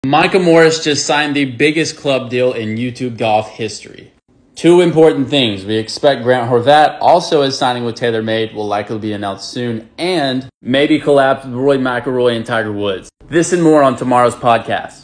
What does it mean moving forward? Let’s find out!
Micah Morris Golf announced News today he is signing with TaylorMade Golf. What does this mean? Will he collab with Tiger woods and Rory? Let chat about it!